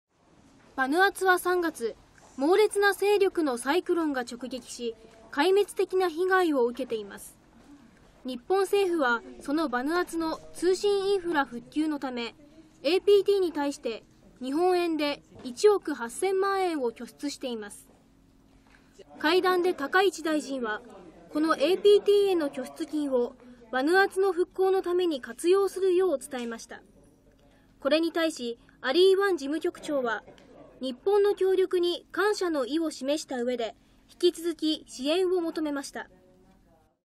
“壊滅的被害”バヌアツ復興に日本の拠出金活用を(15/05/01)
高市総務大臣はバンコクで、APT（アジア・太平洋電気通信共同体）のアリーワン事務局長と会談し、サイクロン被害に遭ったバヌアツの通信インフラ復旧に日本からの拠出金を活用するよう求めました。　バヌアツは3月、猛烈な勢力のサイクロンが直撃し、壊滅的な被害を受けています。